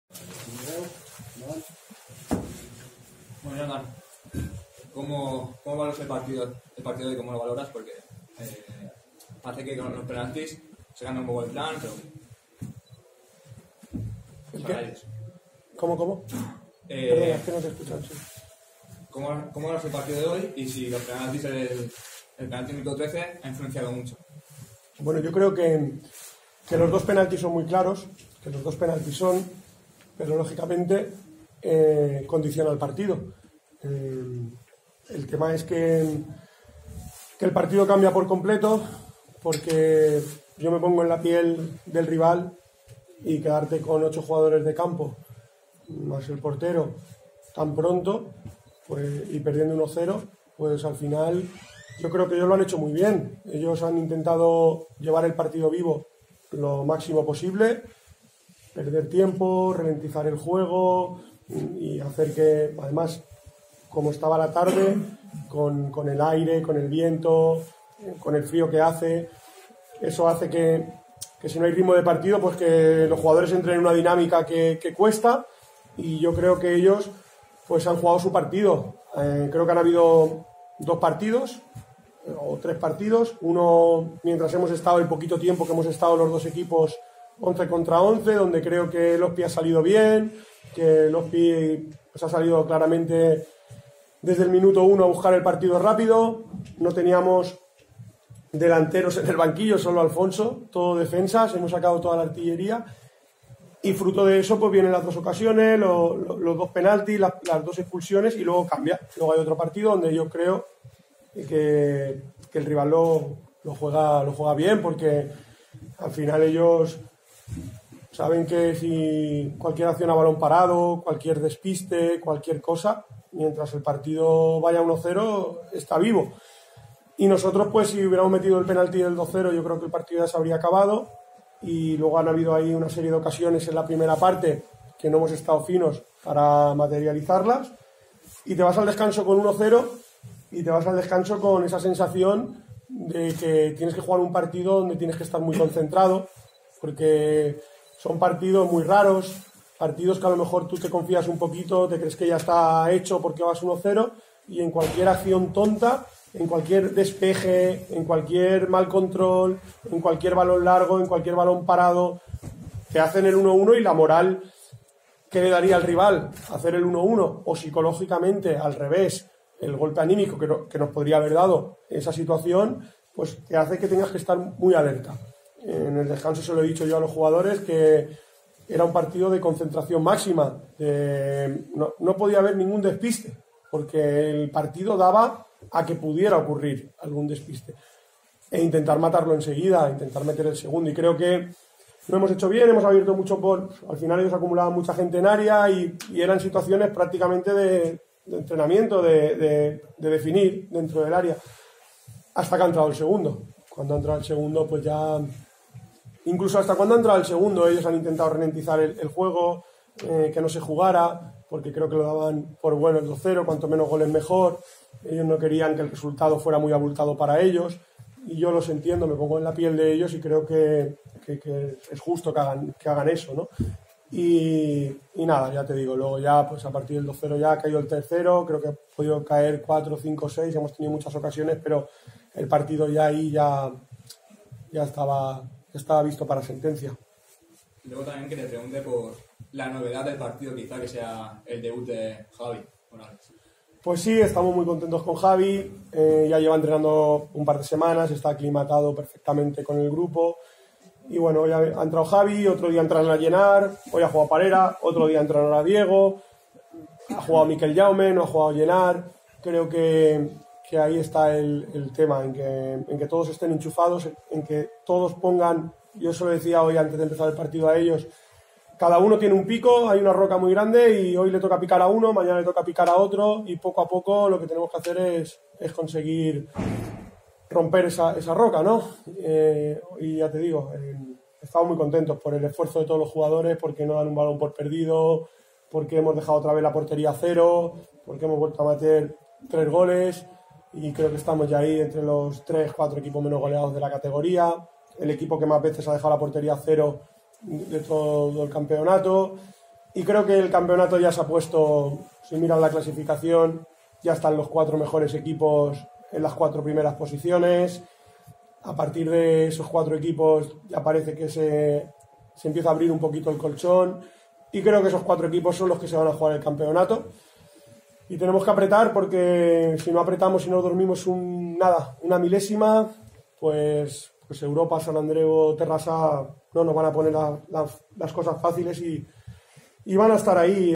Rueda de prensa Jonathan Risueño C. E. L'Hospitalet - U. E. Vilassar de Mar
Rueda de prensa de Jonathan Risueño valorando el partido C. E. l'Hospitalet - U. E. Vilassar de Mar. Partido jugado el día 19/1/2020.